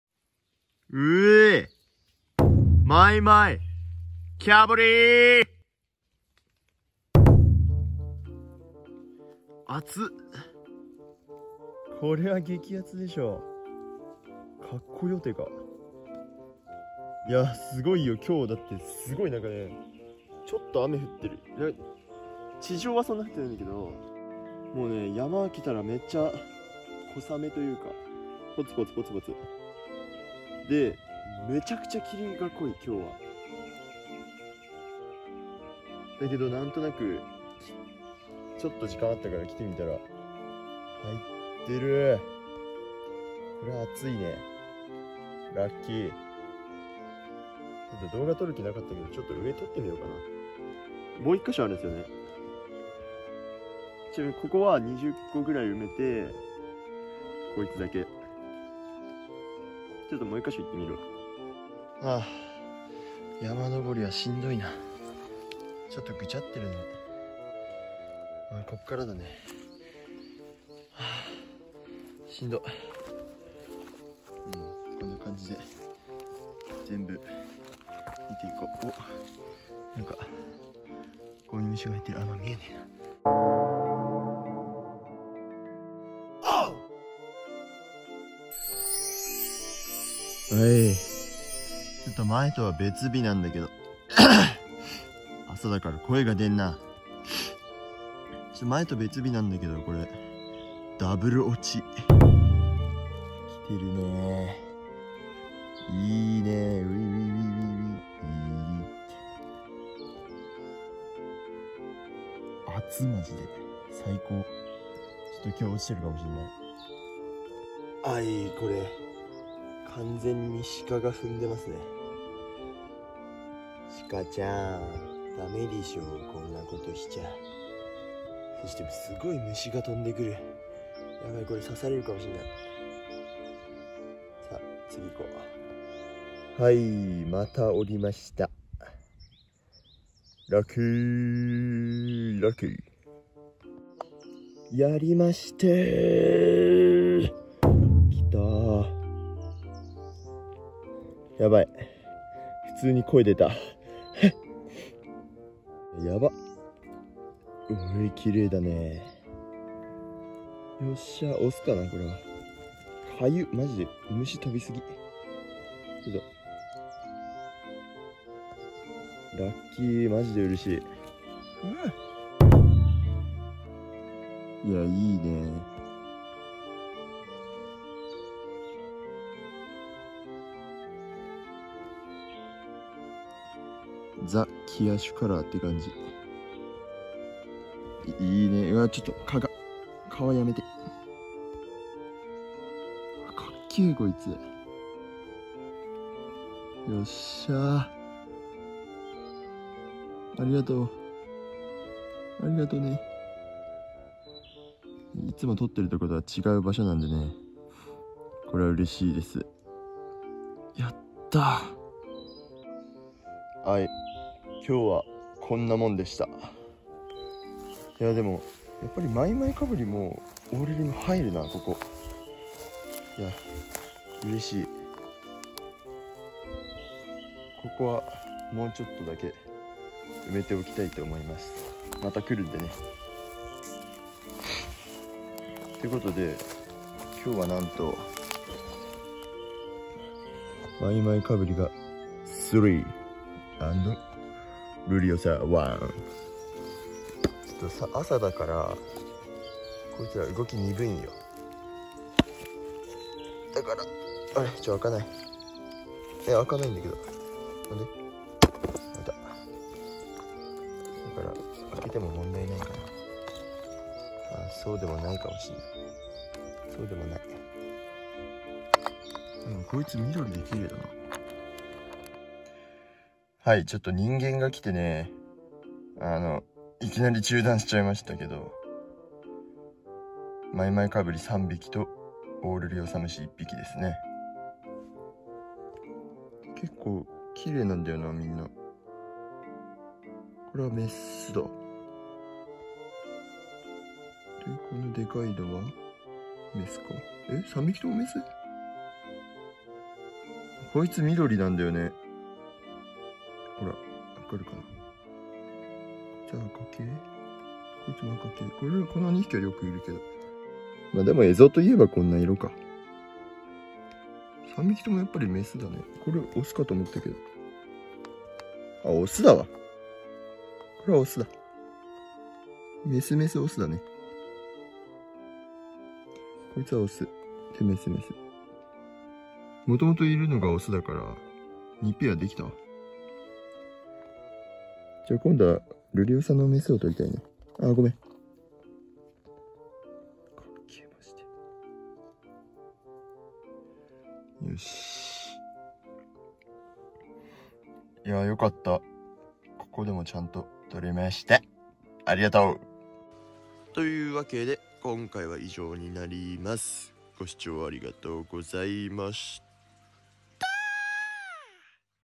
オサムシトラップでマイマイカブリと美しいオサムシを大量ゲット！
これがピットフォールトラップの力です。
最低でも2回同じ場所に行かなきゃいけないのが、トラップ採集の難点ですが
これだけ採れるなら、やる価値ありますね！
ついにマイマイカブリもブリード開始できるな…

BGMは甘茶さんからお借りしました。
効果音は効果音ラボさんからお借りしました。